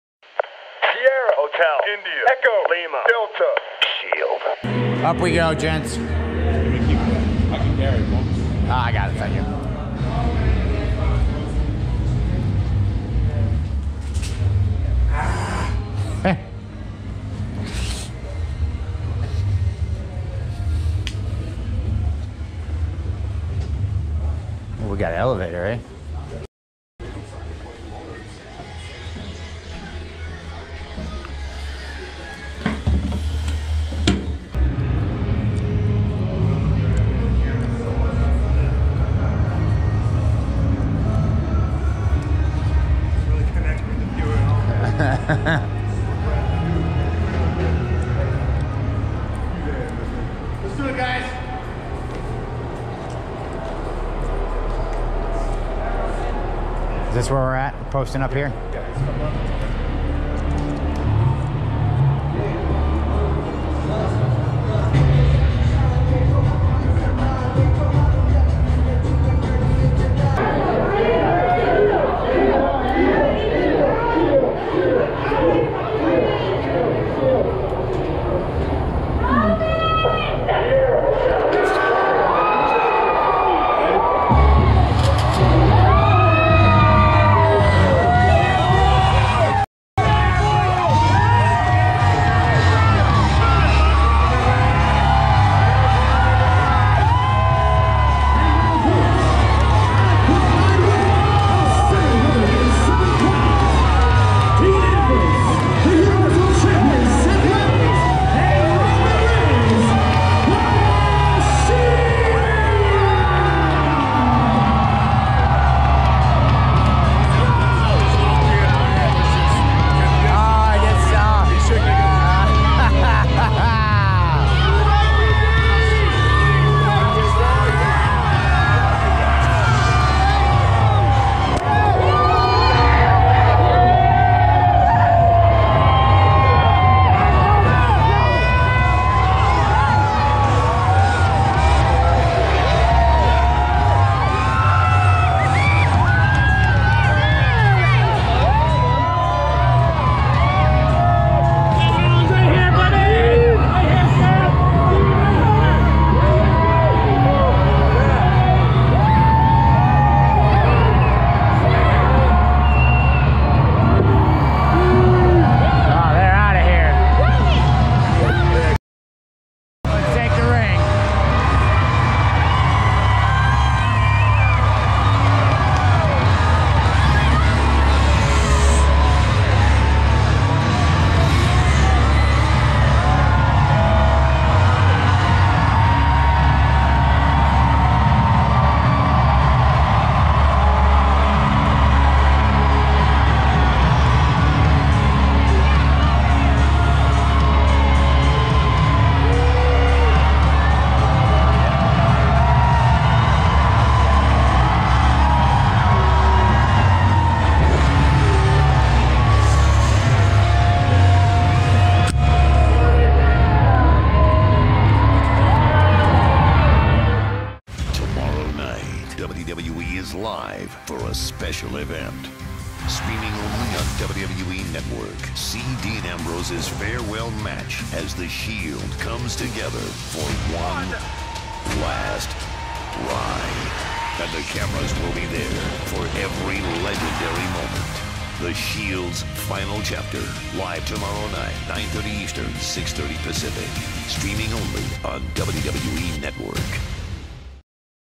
Walk to the ring alongside The Shield: The Shield's Final Chapter Diary
Seth Rollins, Roman Reigns and Dean Ambrose give the WWE Universe a first-person perspective of what it's like for The Hounds of Justice to make their way through the concourse and to the ring for battle.
Follow WWE on YouTube for more exciting action!